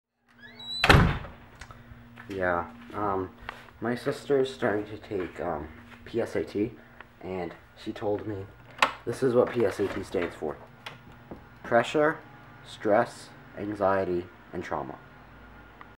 What PSAT Actually Stands For...
For real though, my I watched my sister studying for the SAT and I'm like... "That's gonna be me in, like, two years..."